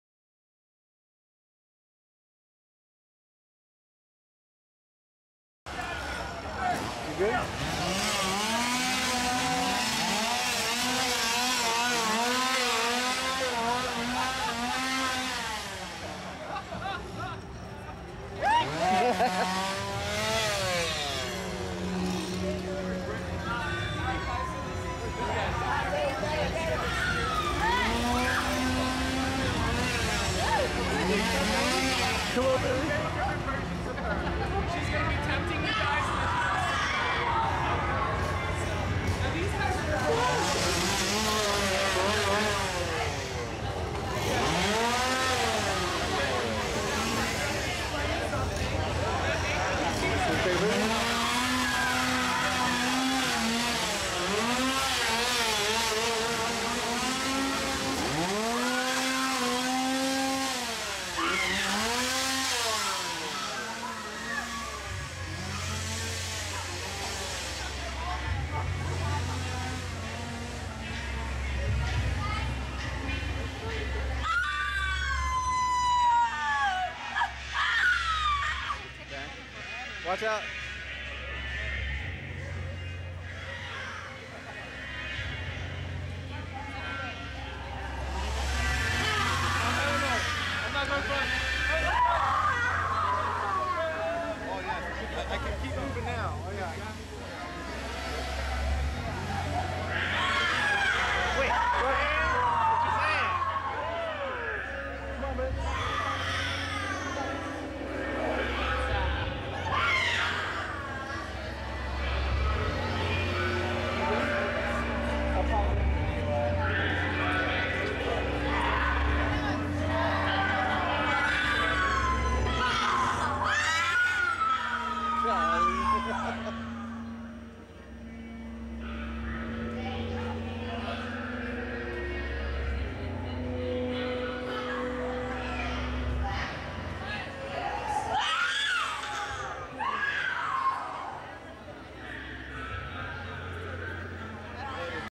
HALLOWEEN HORROR NIGHT 21 YOUR LUCK HAS RUN OUT
Enter if you dare into the lair of the LUCKY! Your choices will determine if you make it out alive. Witness lucks deadly wrath, will she choose you to be her next victim?